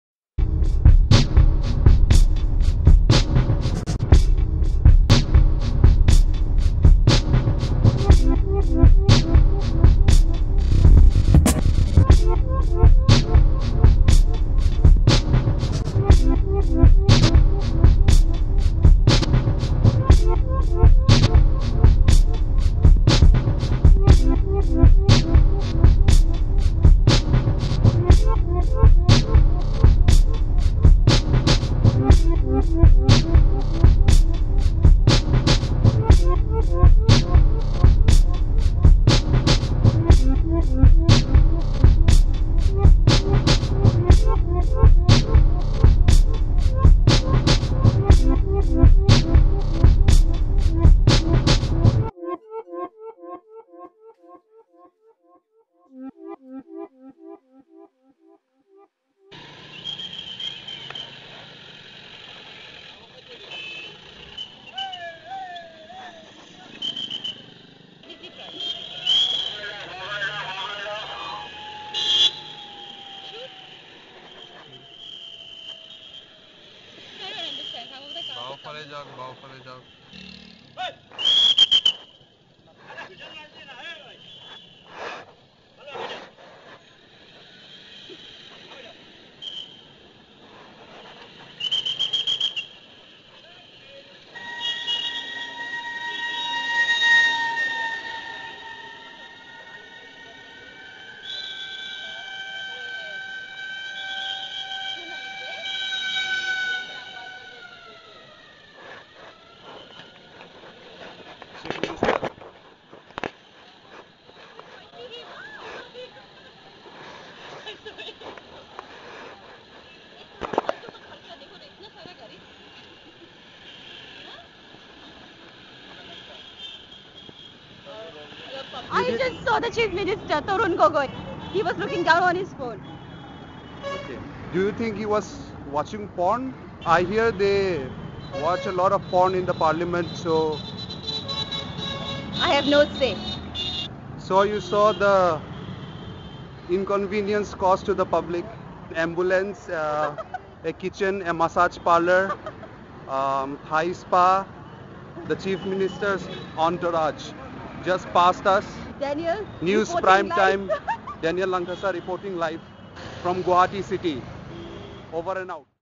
STOPPED BY THE CHIEF MINISTER!
Chief Minister of Assam Tarun Gogoi spotted! Thrift shopping at Fancy Bazaar, Guwahati.

Who ar